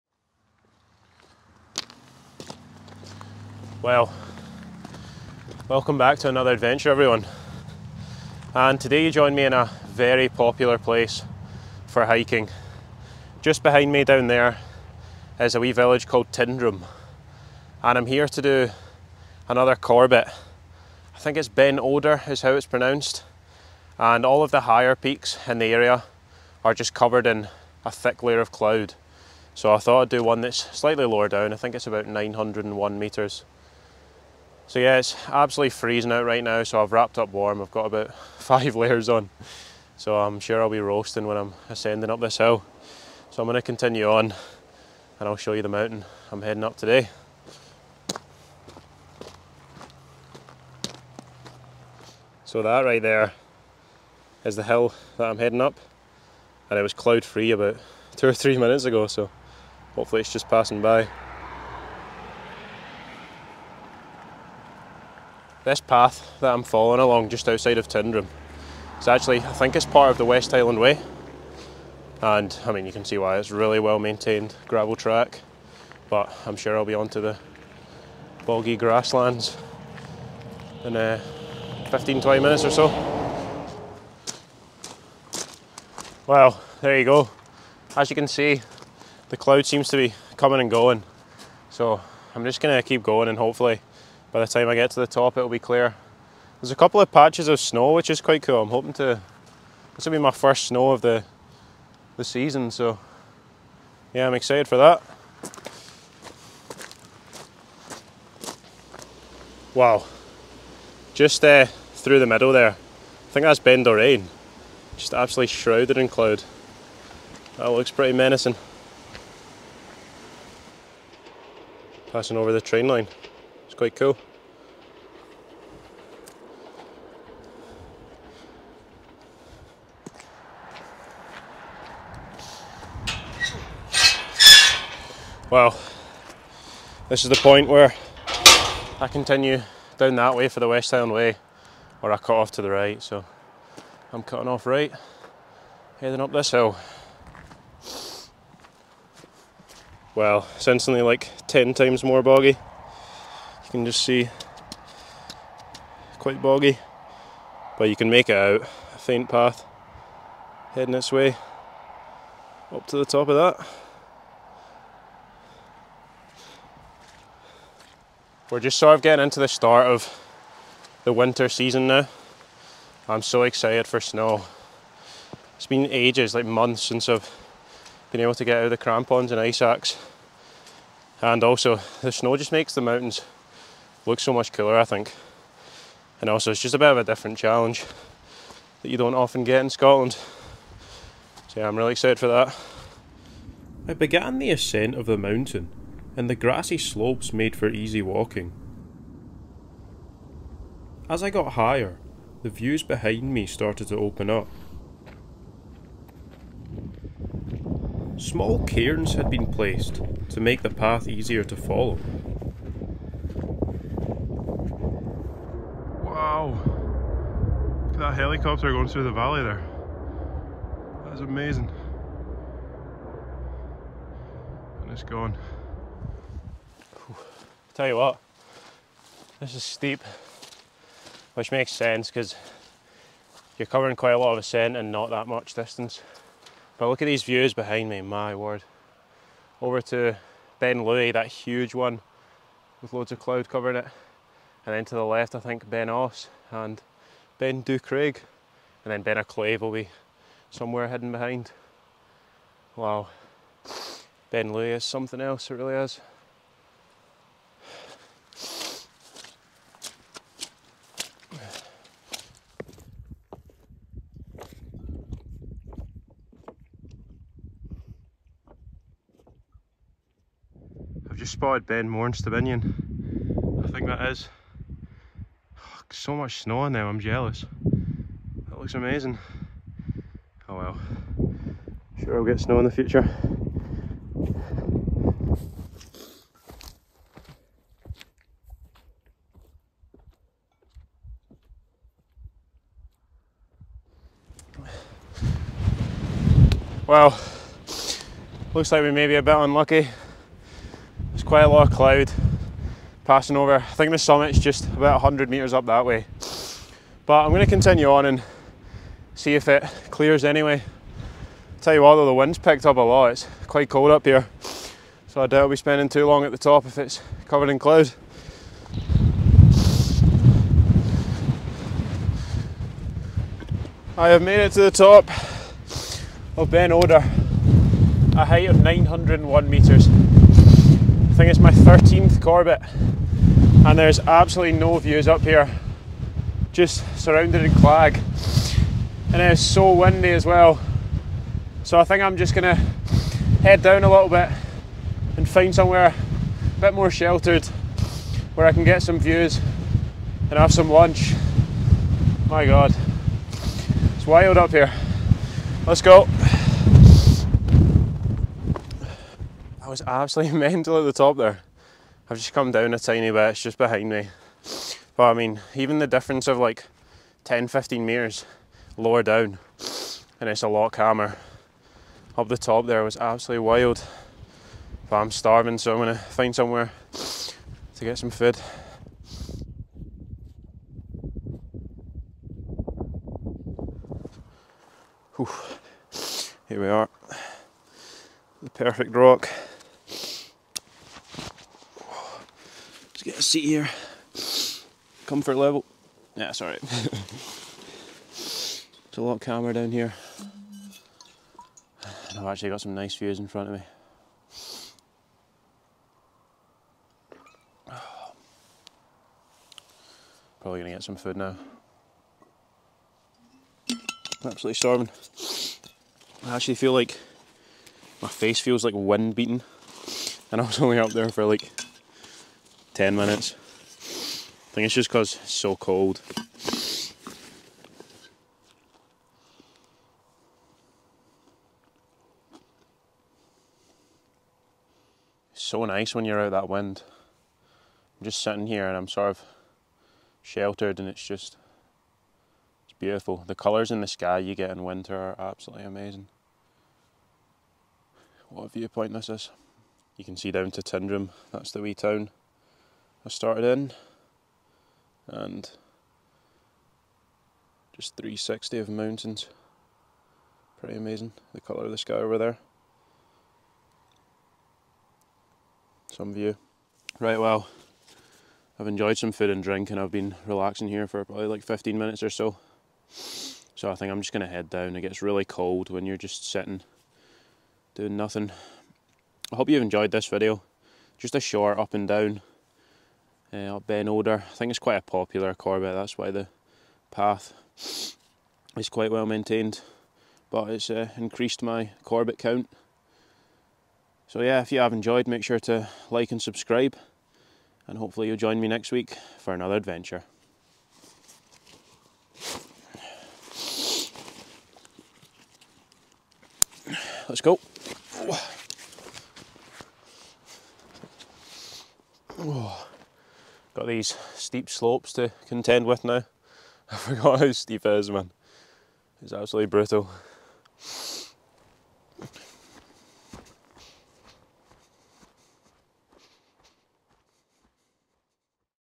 A Short Hike Up A Mountain In The Scottish Highlands
You join me just outside a small town (Tyndrum) in the Scottish highlands. I planned to climb one of the mountains in the area and it ended up being a bit easier than I expected. Hope you enjoy